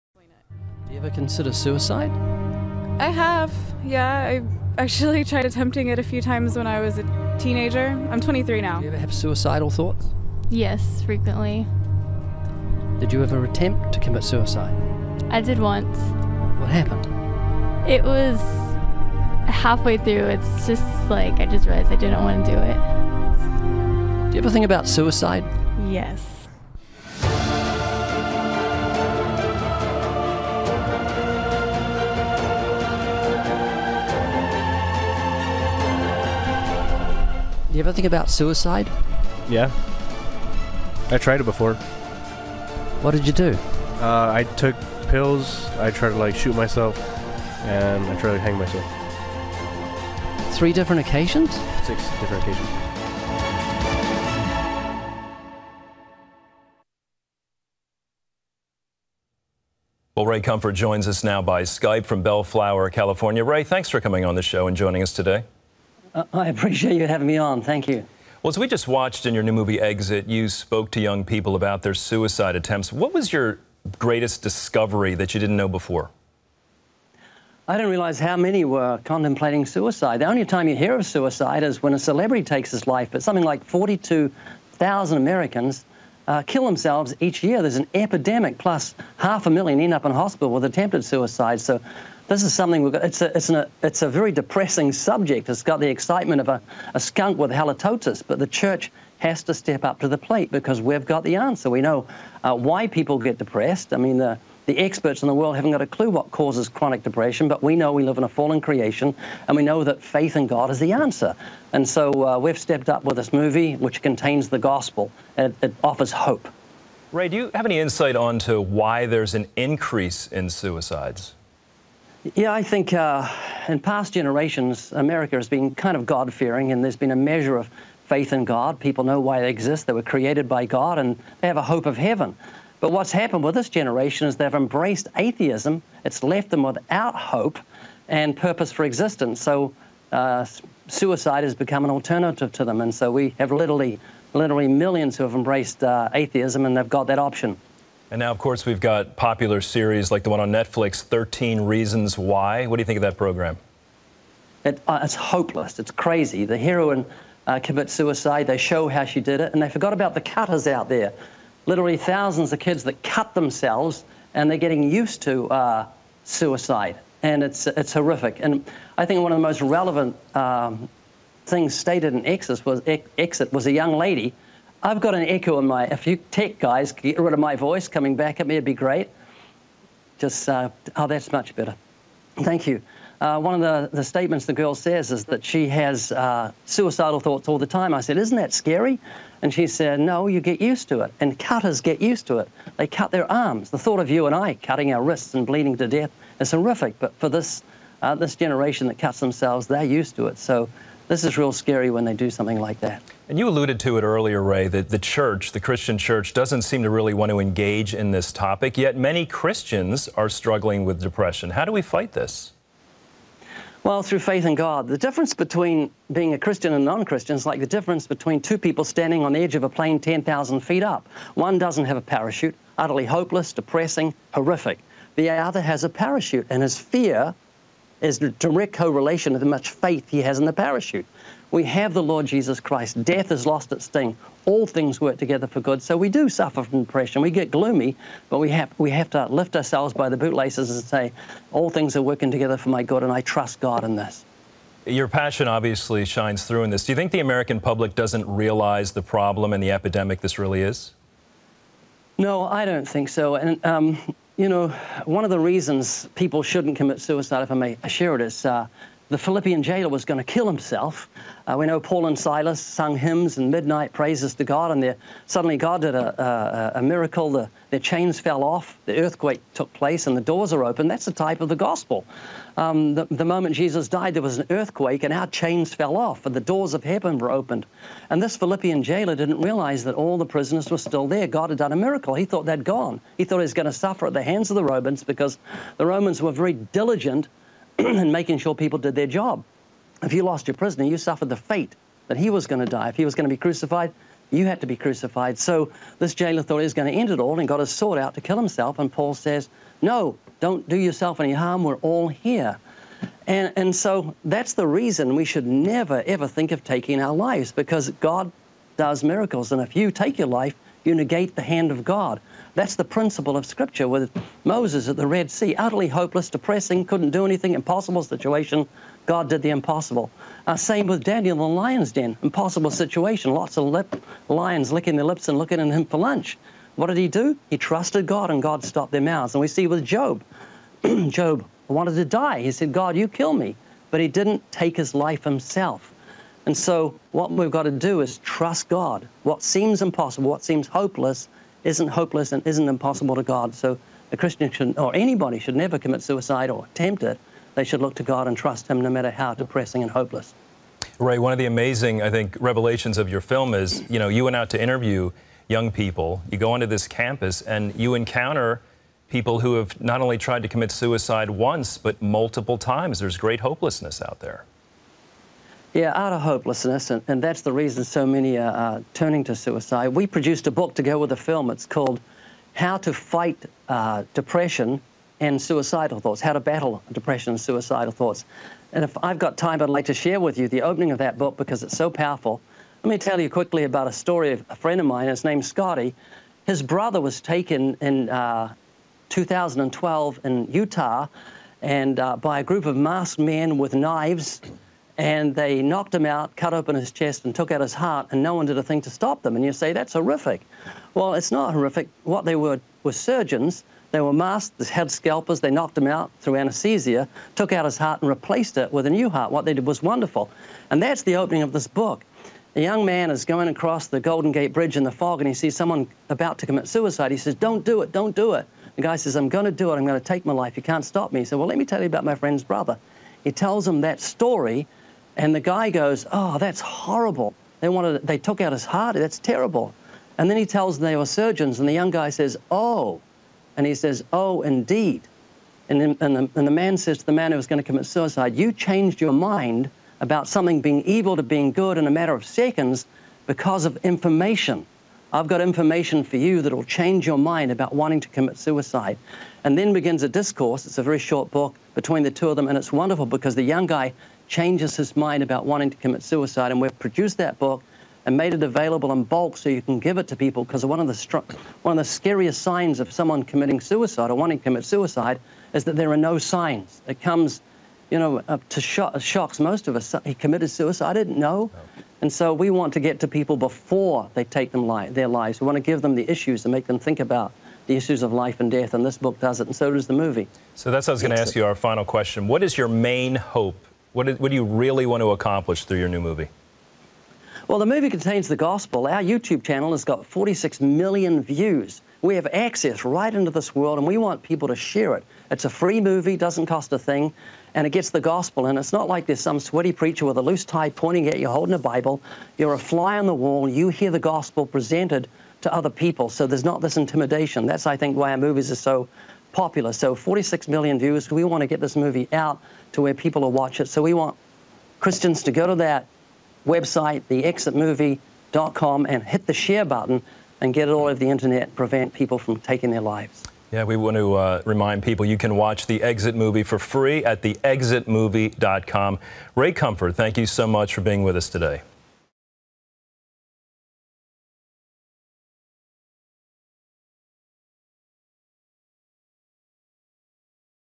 Hope For Those Struggling with Depression
Filmmaker Ray Comfort believes his latest project, Exit: The Appeal of Suicide, will inspire hope in those struggling with depression.